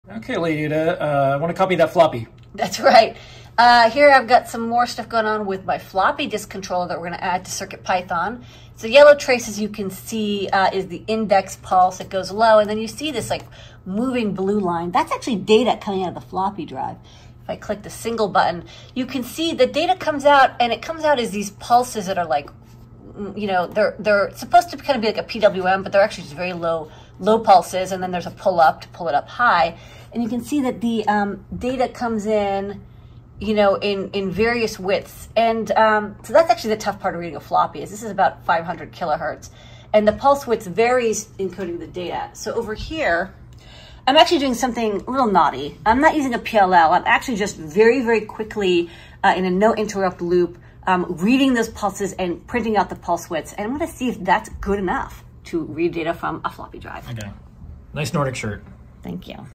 Adding floppy disk support to CircuitPython, step 2
OK now that we've gotten the motor up and running, and the drive selected in step 1 - we know our wiring is correct. step 2 is getting data out! unlike almost all protocols we're used to, there's no clock pin on floppy data. it comes out in MFM format (at least, this floppy disk format does) which means we get a series of ~500kHz open-drain pulses with short negative widths, and varying positive widths that indicate flux transitions. now the *proper* thing to do here is use a PLL to sync up with the pulses since the motor speed can vary a few %, then convert the pulse widths to bits of data. but we're gonna do it dirty to start, and just busy-poll the pin using a fast register gpio read and collect pulse widths, then maybe decode and bitpack them on the fly. not sure if this is workable yet, but it looks OK so far: just started collecting the data pulses and they seem to be binnable.